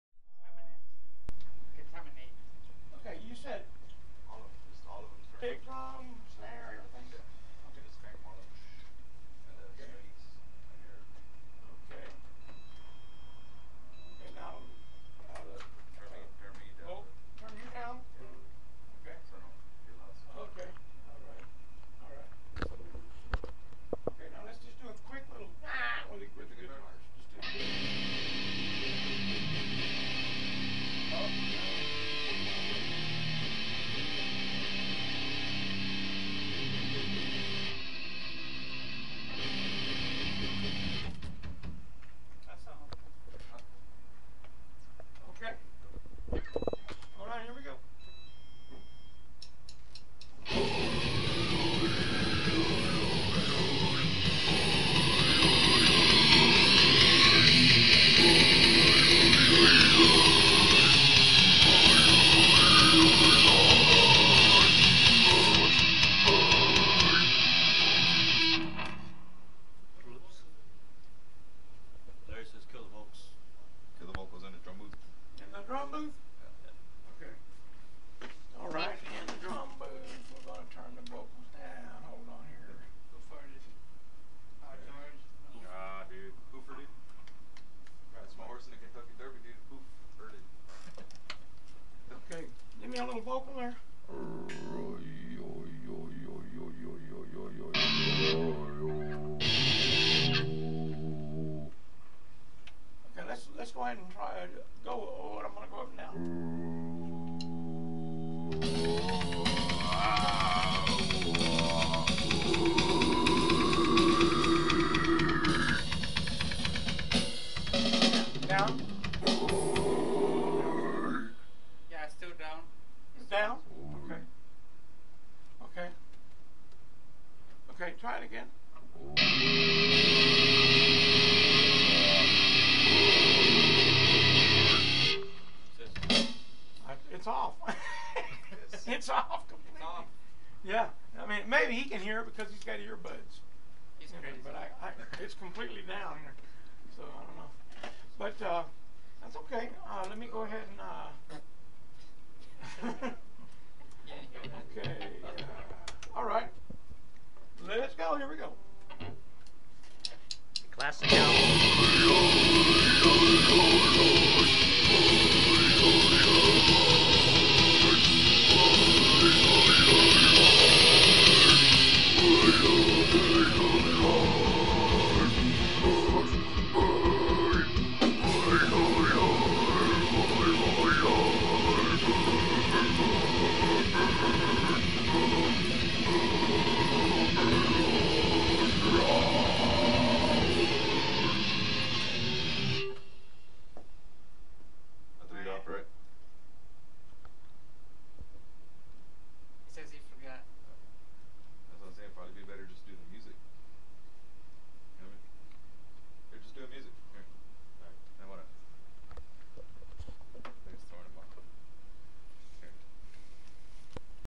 Grotesque Formation 2.MP4
Grotesque Formation recording thier music at Gilleys Studio in Corpus Christi,Texas on 7 19 11!